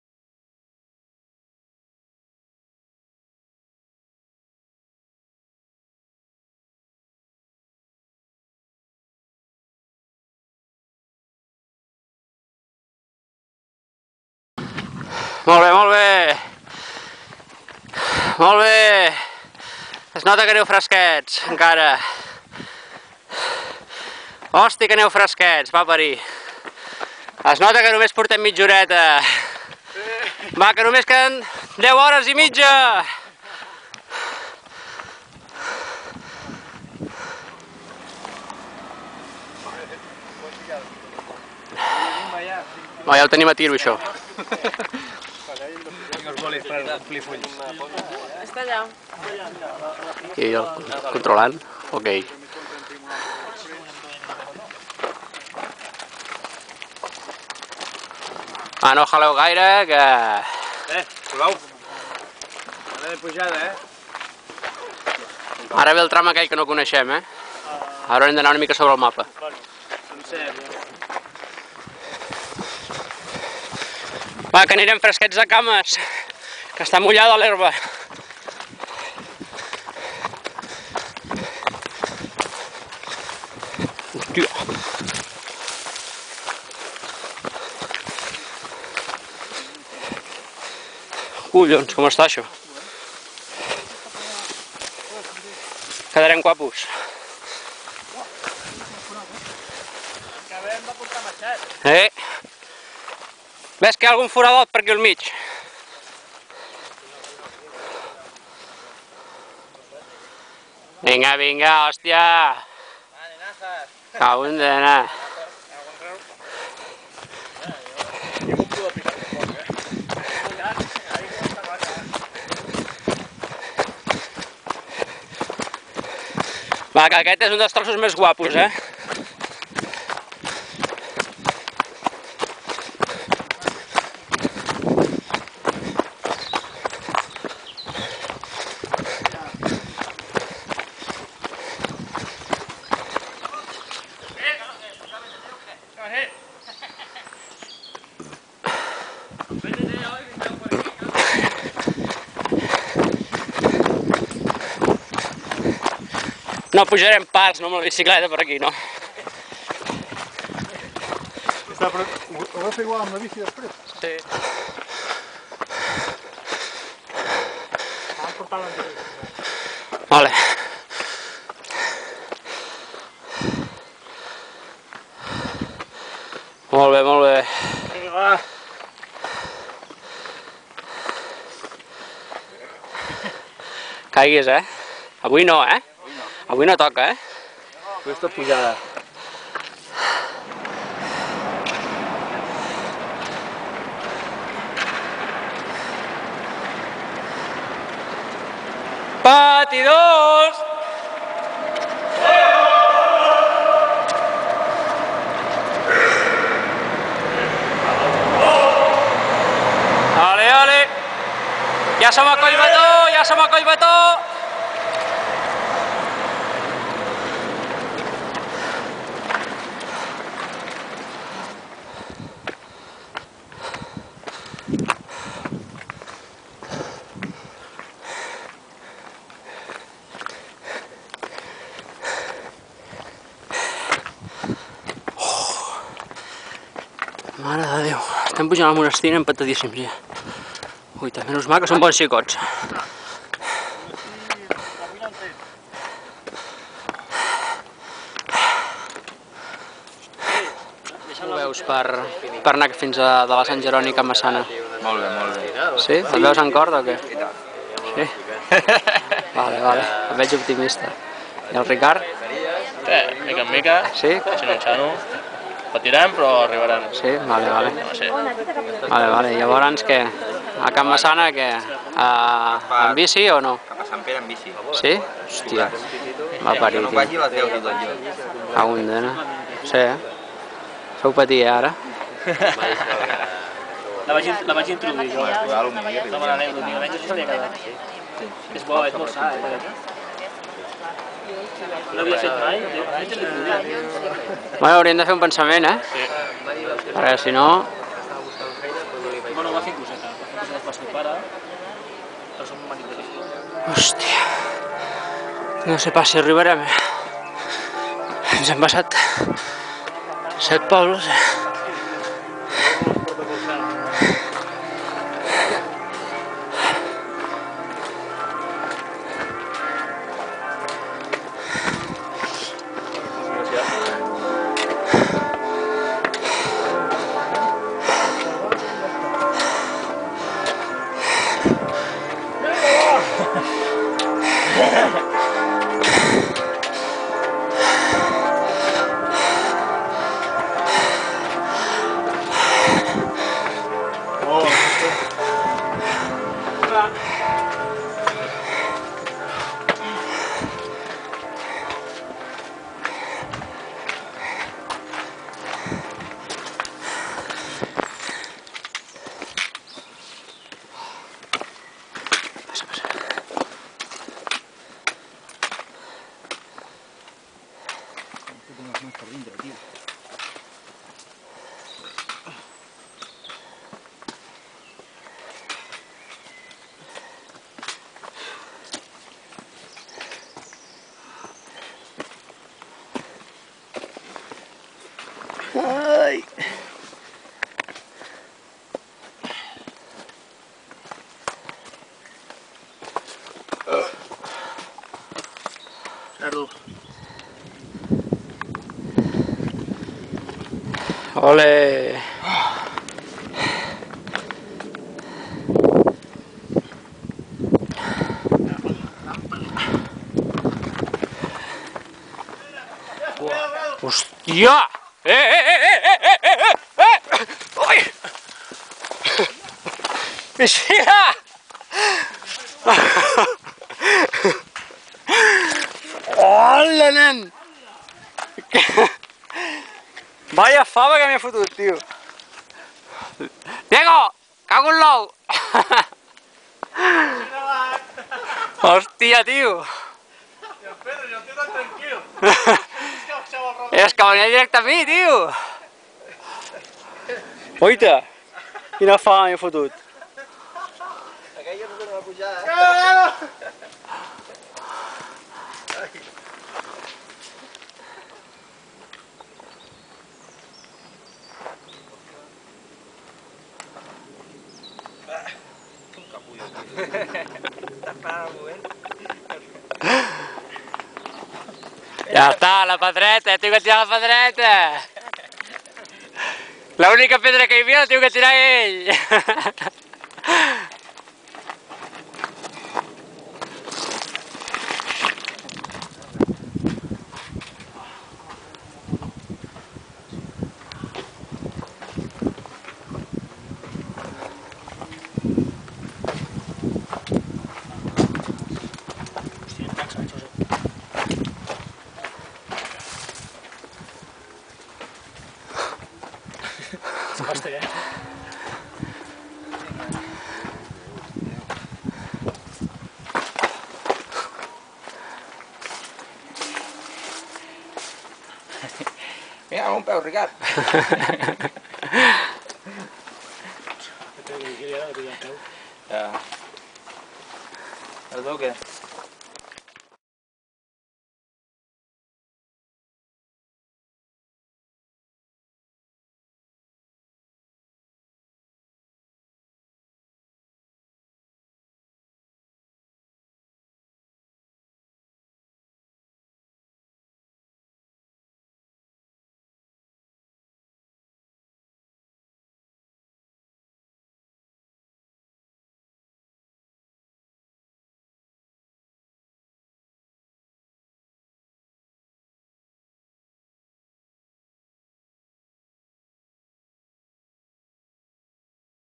ULTRA DuaTRAIL Patidors "La Penitència"
Torrelavit - Montserrat - Torrelavit
75 km / 5200m D+ / NON-STOP
30 km córrer
10 km trail
35 km btt